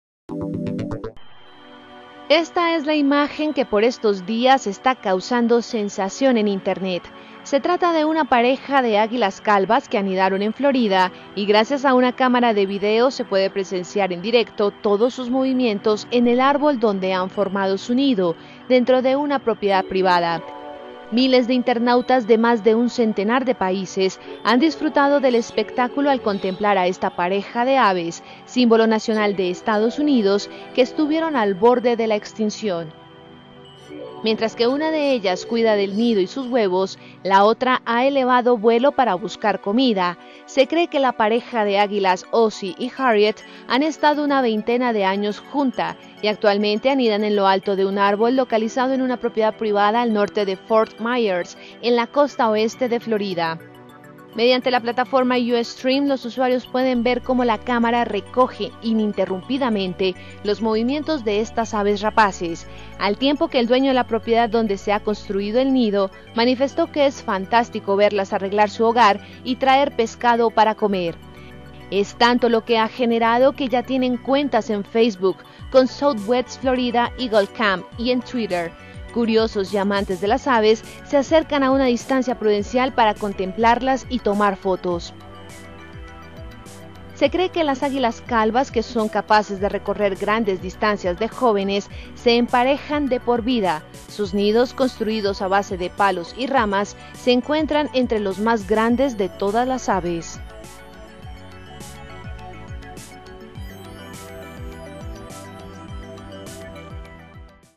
Nido de una pareja de águilas calvas causa sensación en internet
Una pareja de águilas calvas, conocidas por su cola y cabeza de plumas blancas, es el nuevo fenómeno de internet, gracias a una cámara de vídeo que recoge en directo todos sus movimientos en el árbol donde anidan, en una propiedad privada de Florida.